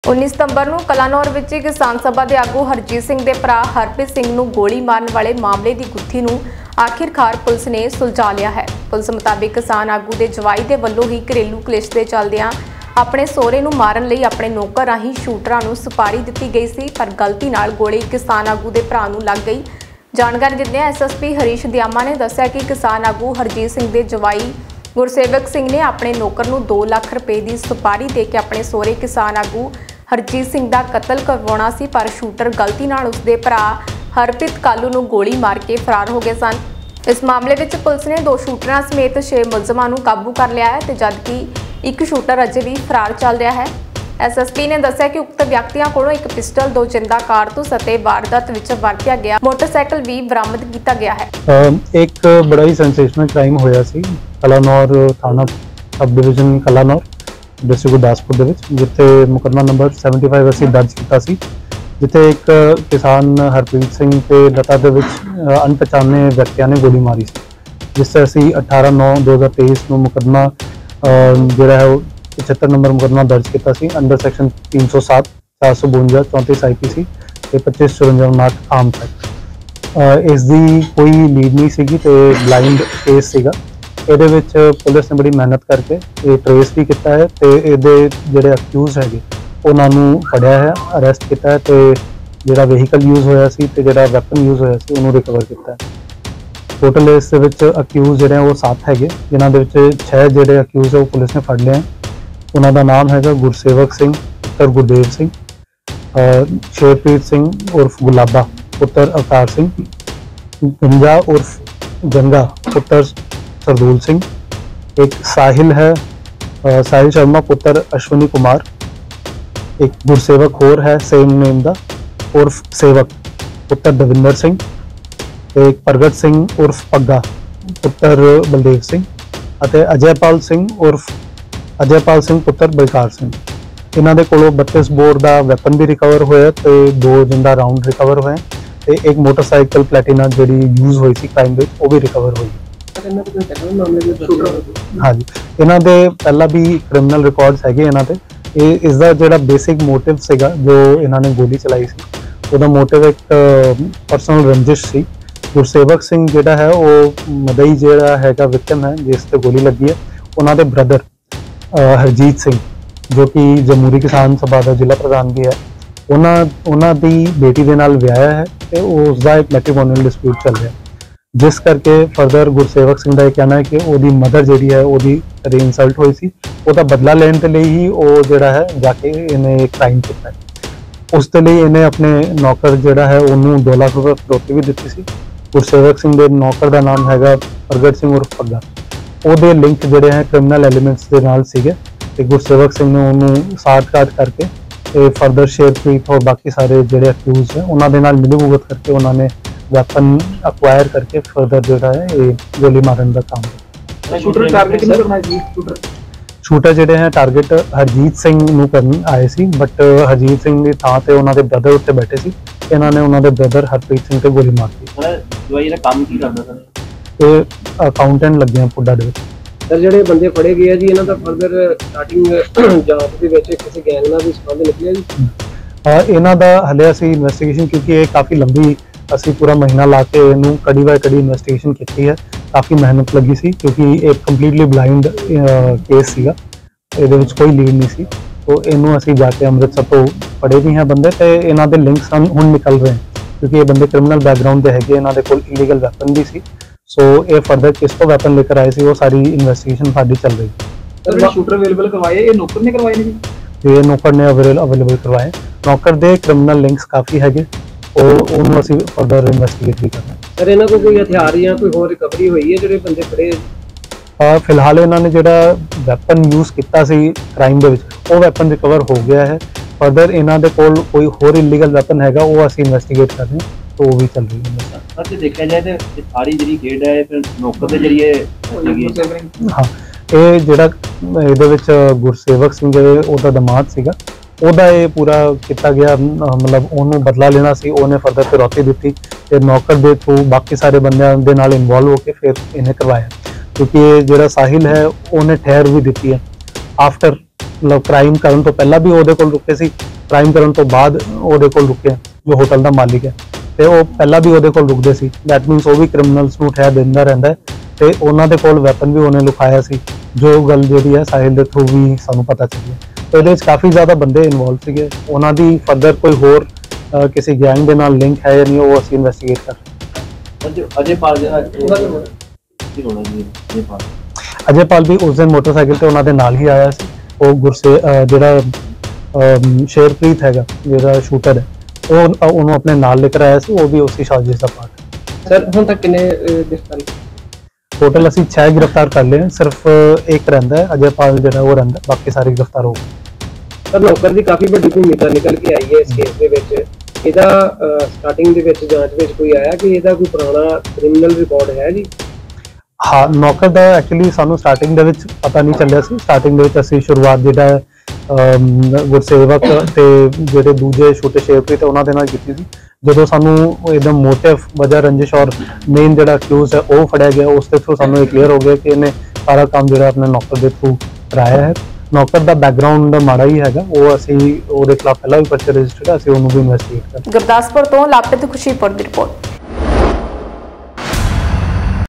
किसान नेता हरजीत के भाई हरप्रीत को गोली मारने वाला मामला पुलिस ने सुलझाया
कलानौर में किसान सभा के नेता हरजीत सिंह के भाई हरप्रीत सिंह को गोली मारने वाला मामला पुलिस ने सुलझाया, एसएसपी हरीश दयामा ने दी जानकारी। किसान नेता हरजीत के दामाद ने घरेलू कलेश के चलते ससूर की हत्या के लिए अपने नौकर के जरिए शूटरों को दी थी सुपारी। लेकिन, गलती से गोली किसान नेता के भाई को लगी, पुलिस ने 2 शूटरों सहित 6 आरोपियों को किया काबू व फरार शूटर की तलाश जारी। आरोपियों से 1 पिस्टल, दो जिंदा कारतूस व एक मोटसाइकिल बरामद। गुरदासपुर से लवप्रीत खुशीपुर की रिपोर्ट। 3 नवंबर 2023